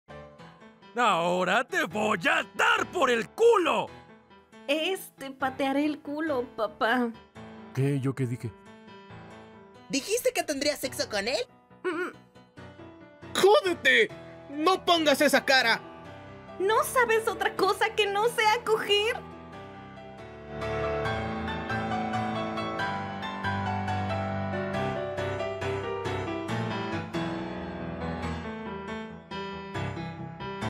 Lucifer Le quiere dar a Adán Hazbin Hotel Cómic en ESPAÑOL
VA
Adán @fukudubs
Lucifer @DaJonchoDubs
Angel Dust @MIKER_DOBLAJES
Charlie @deifandubs

CRÉDITOS

EDICIÓN DE VIDEO Y MEZCLA DE SONIDO